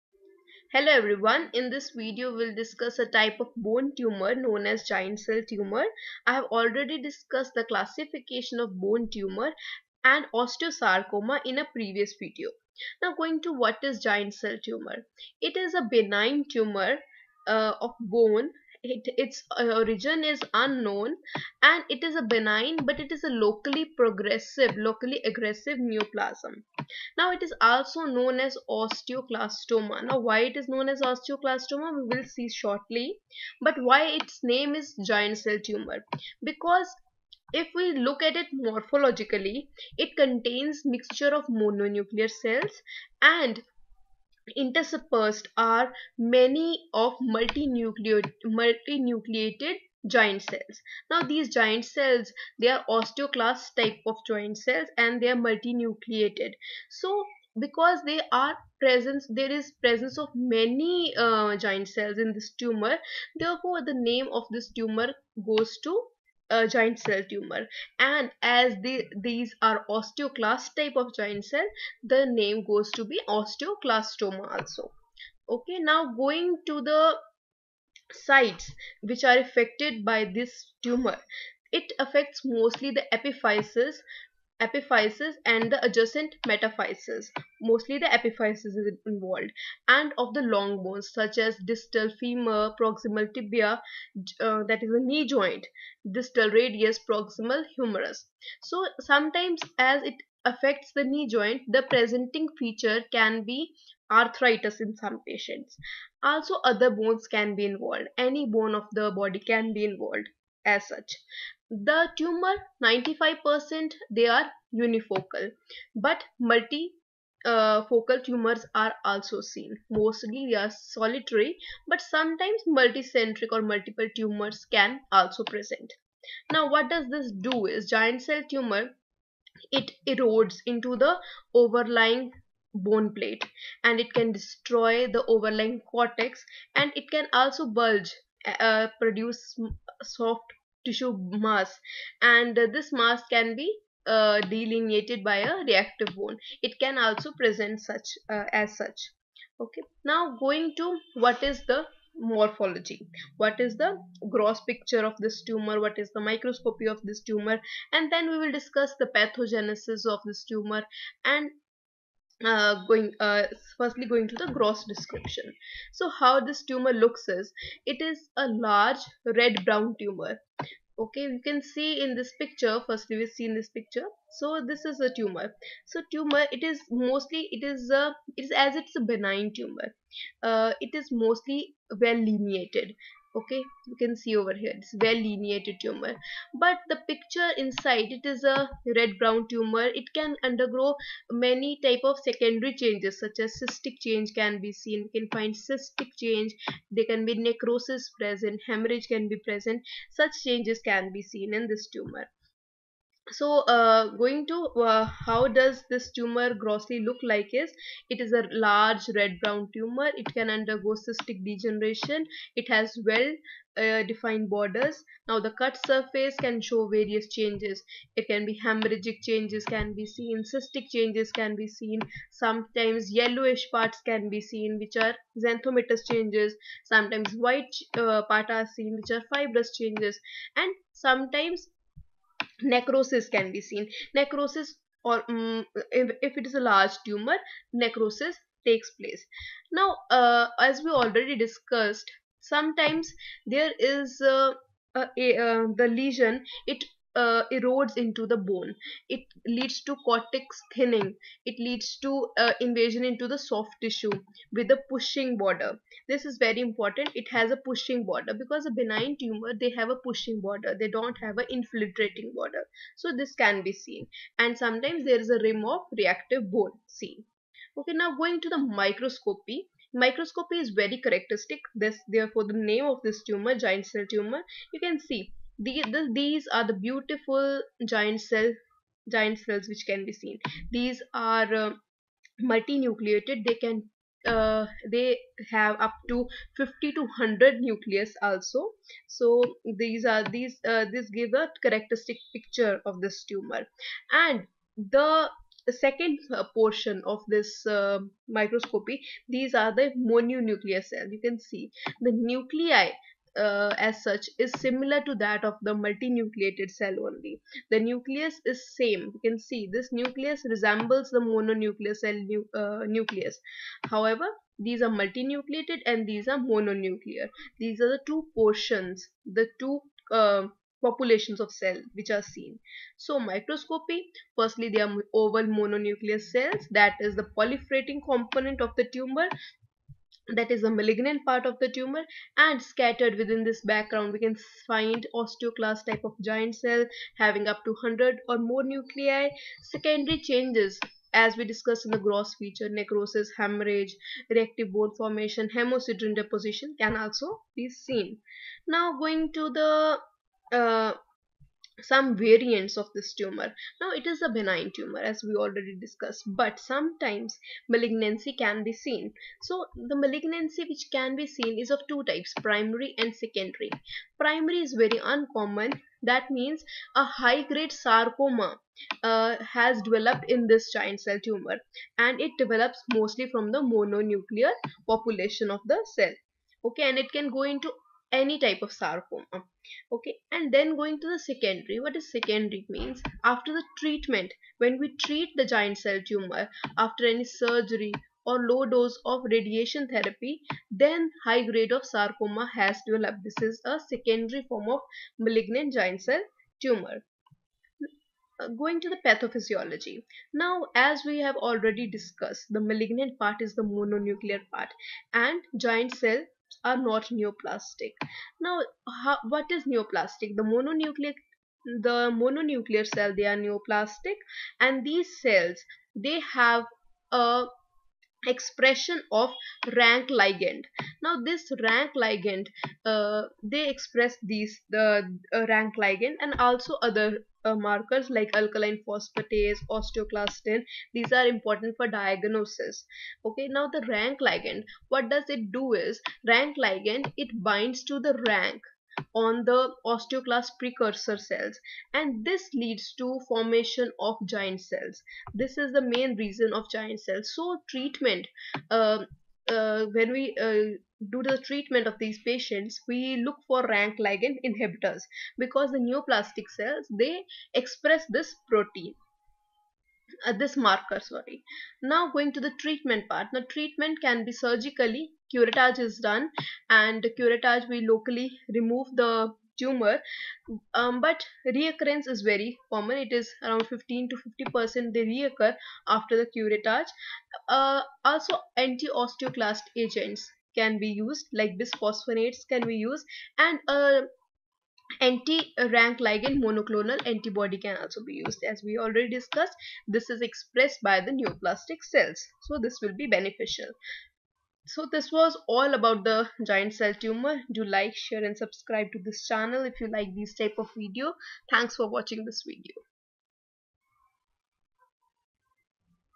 Giant Cell Tumor: Morphology, Pathogenesis
Giant-cell tumor is so named because it contains a mixture of mononuclear cells and a profusion of multinucleated osteoclast-type giant cells, giving rise to the synonym osteoclastoma